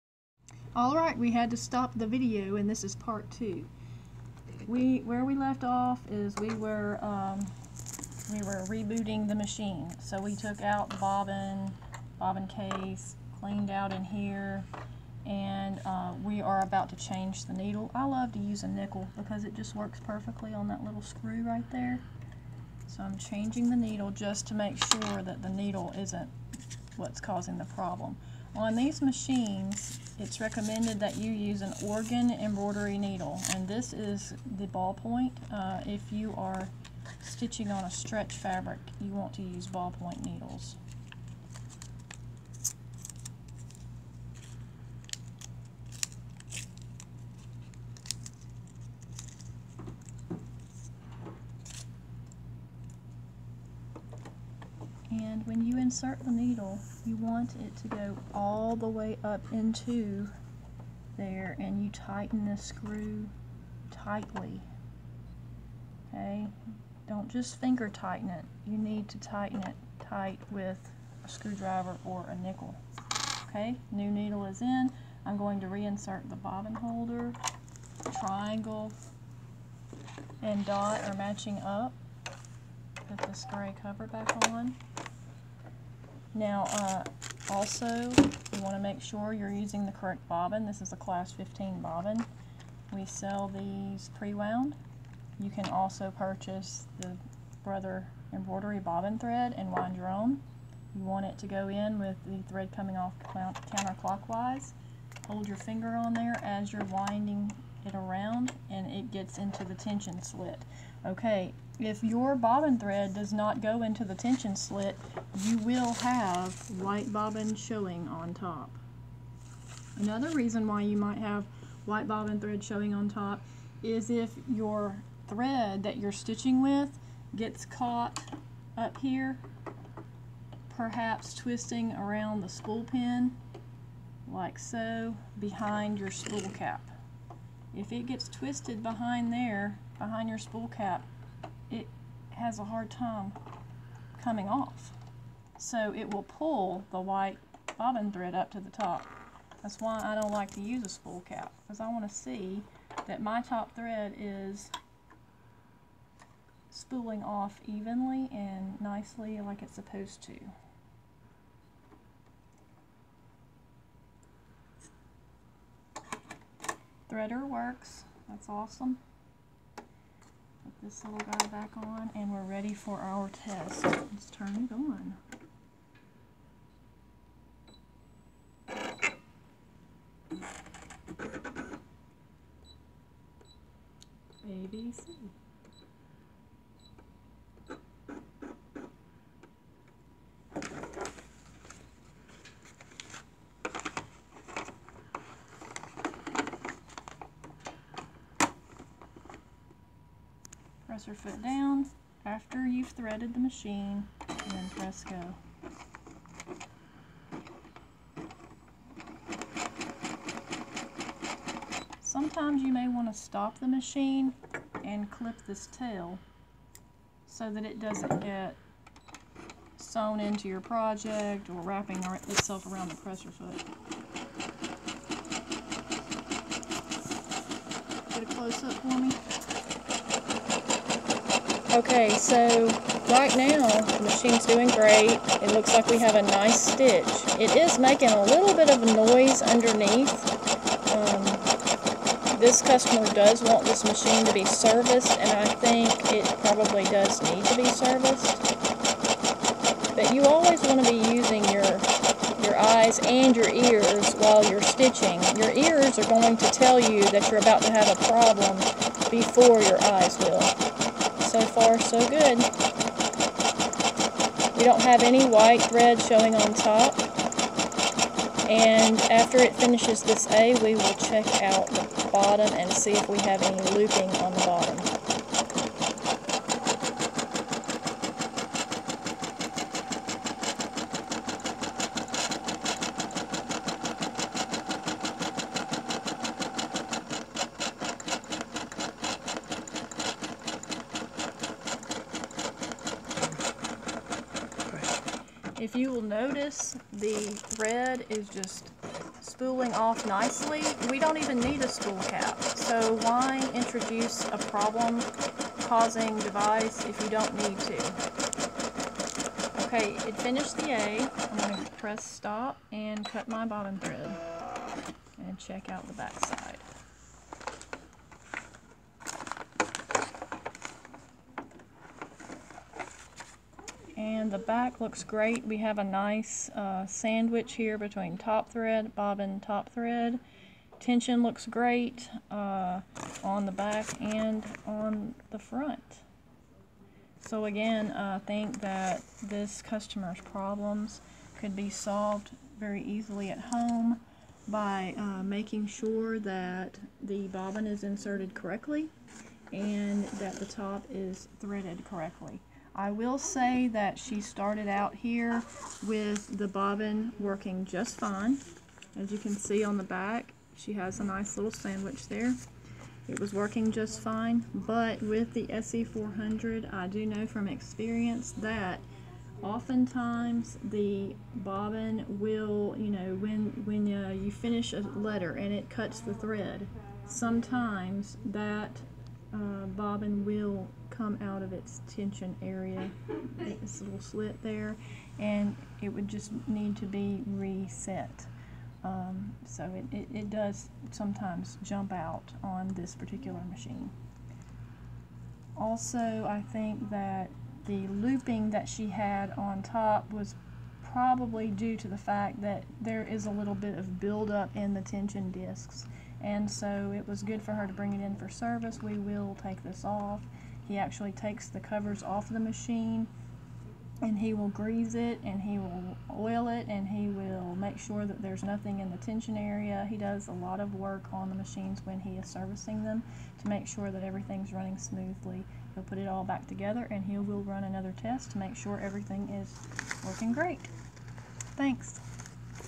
SE400 Troubleshoot 5/10/17 Part Two
Tension problems corrected with proper threading.